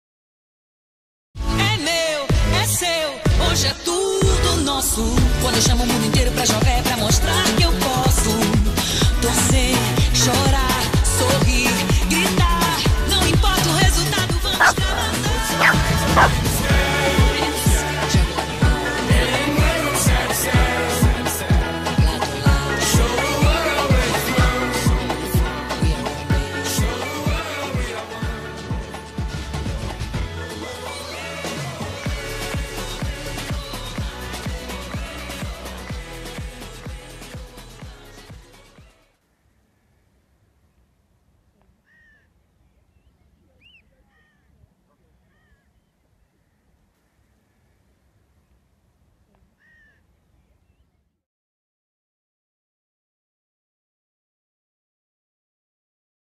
Dog predicts World Cup 2014 results
Watch this video to see Izo (aka @TheIzoTimes) predicting the next World Cup Winner Is...